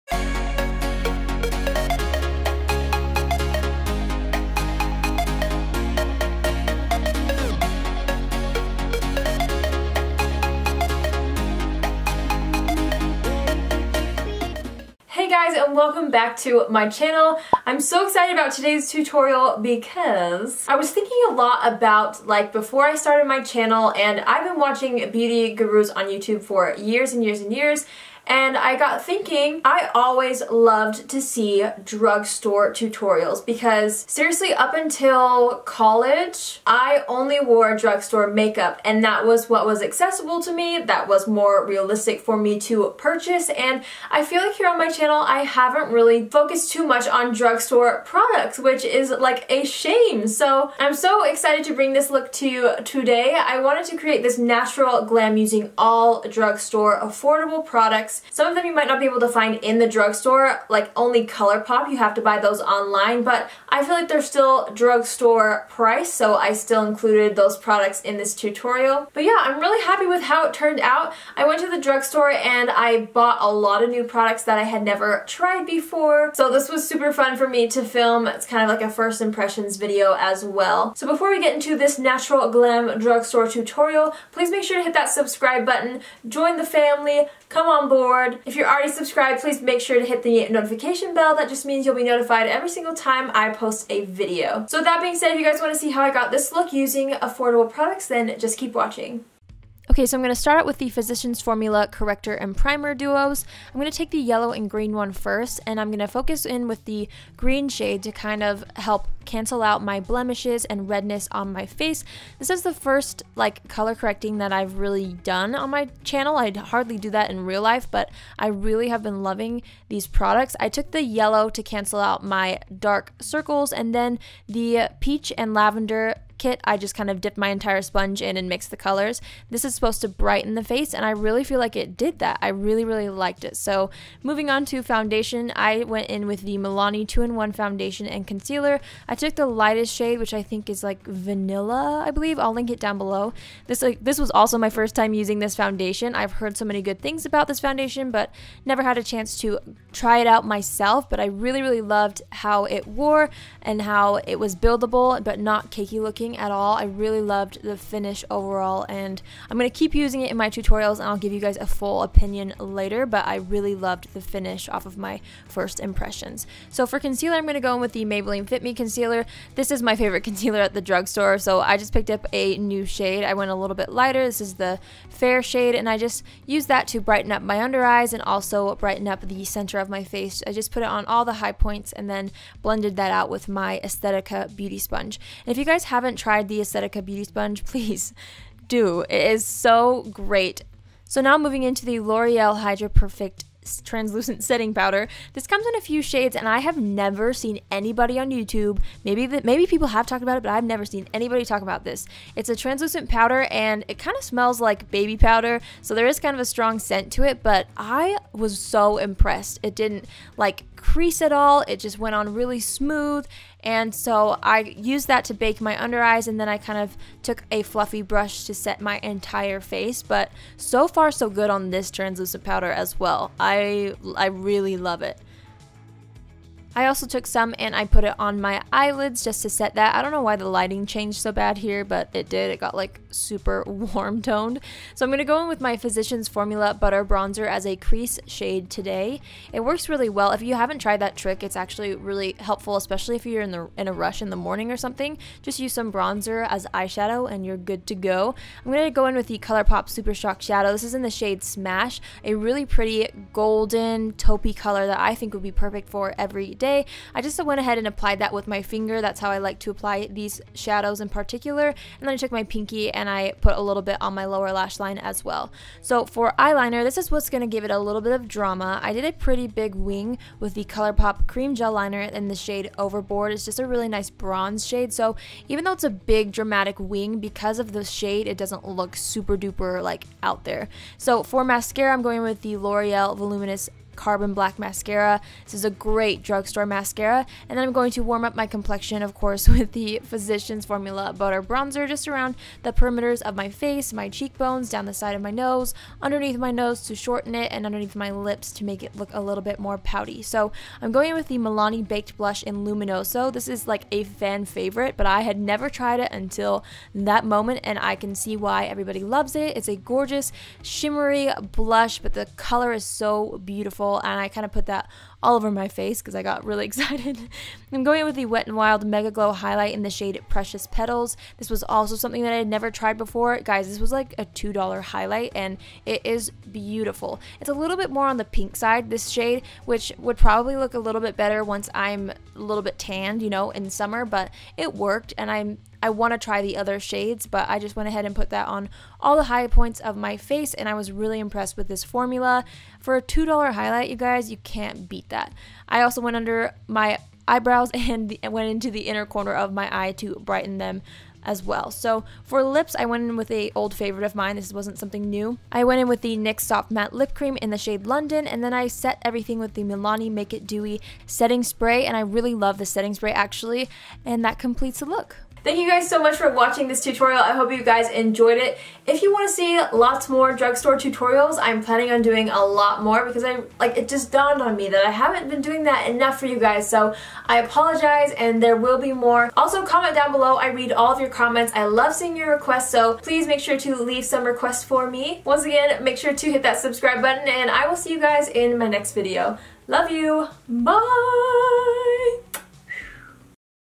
TRYING NEW DRUGSTORE PRODUCTS | NATURAL GLAM | ALLIE G BEAUTY
Hey loves! I went to the drugstore earlier this week and picked up some products that I have never tried before! I had so much fun creating this affordable drugstore natural glam makeup look :) I’ll be posting many more drugstore looks in the future!

Maybelline Fit Me! Concealer in Fair

Wet n’ Wild Megaglow Highlighter in “Precious Petals” (It was on sale for $2 at my Walmart!)

CAMERA USED:

Canon 80D

* Use the code "alliegbeauty" for a FREE Loving Tan Deluxe Applicator Mitt with any mousse purchase